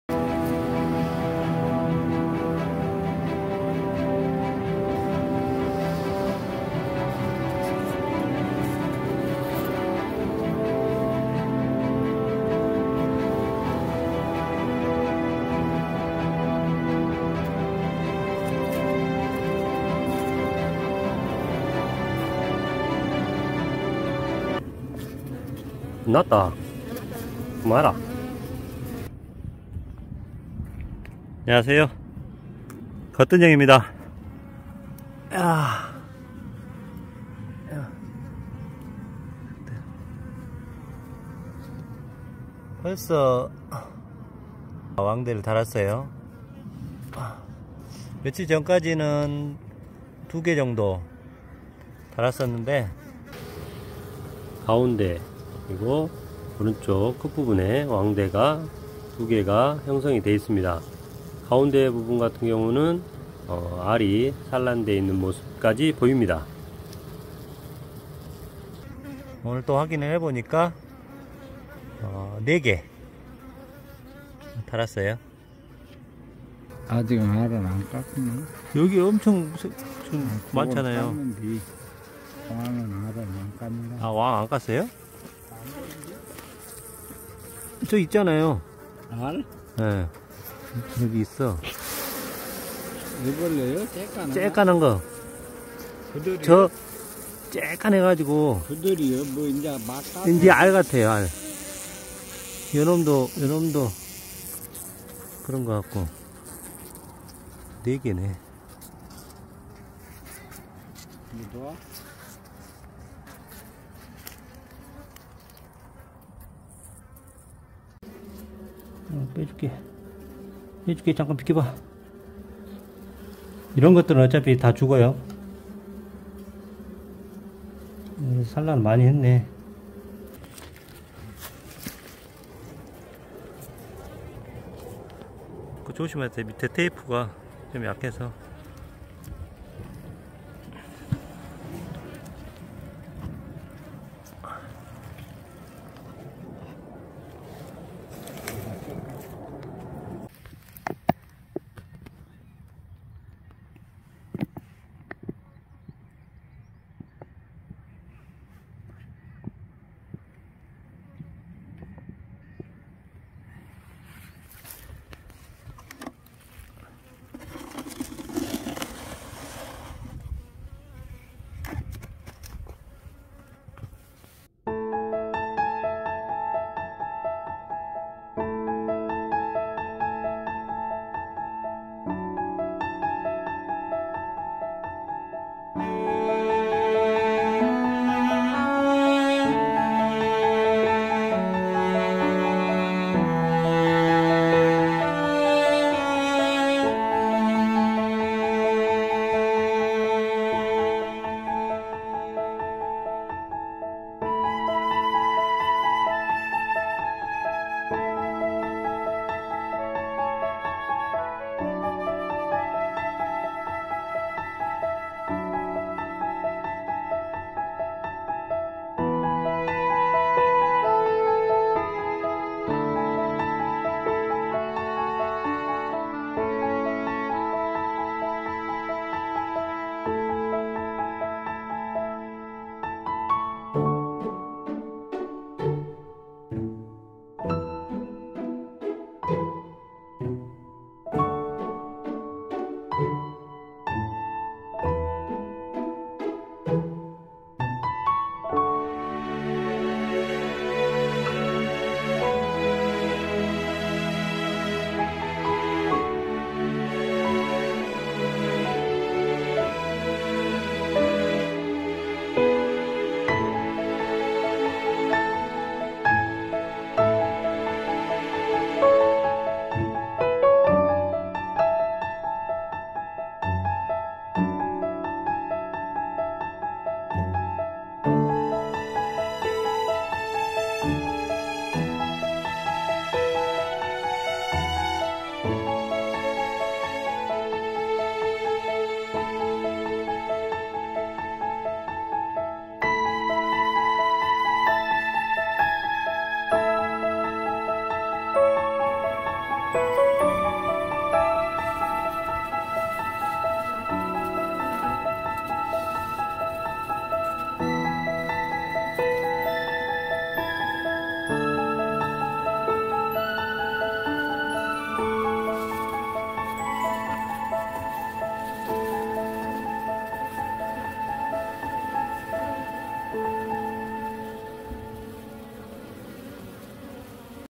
왕대가 4개? 꿀벌들아! 4통 분봉 가즈아!^^
꿀벌들의 생각보다 이른 분봉준비~
내검중에 발견된 4개의 왕대~
올해 4통분봉을 위해 좋은 곳으로의 자리이동과 먹이를 보충해 주는 영상입니다.^^토종벌 분봉 꿀#